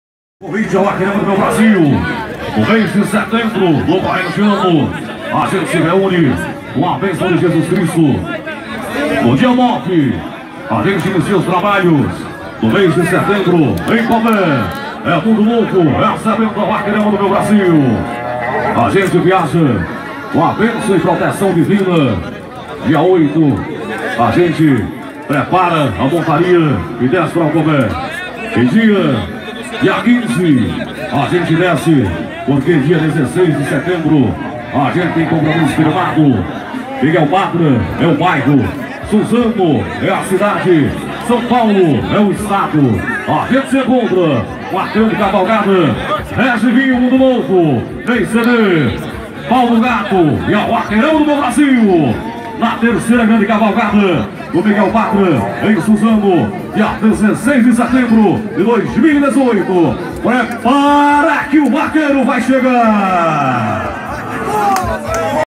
Cavalgadas na BA e SP
No último domingo, 07 de janeiro de 2018, o locutor Marcelo Augusto divulgou as datas das cavalgadas de Mundo Novo-BA e de Suzano-SP.

A confirmação aconteceu no Rancho 2 Irmãos, durante o percurso da 13ª Cavalgada de Umbuzeiro, no município de Mundo Novo-BA.

A 10ª Cavalgada da Amizade, organizada por Fabiano Lima, será realizada no dia 09 de setembro de 2018. Este ano sairá do povoado do Cobé, para o Parque de Exposição de Mundo Novo.

A 3ª Cavalgada do bairro Miguel Badra, em Suzano-SP, organizada pelo mundonovenses Regi do Vinho e Ney CD,s, será realizada no dia 16 de setembro de 2018.

Os dois eventos terão locução de Marcelo Augusto, cobertura do Blog Agmar Rios pela terceira vez e show de Silvaney e Robson, com outras atrações que ainda serão divulgas.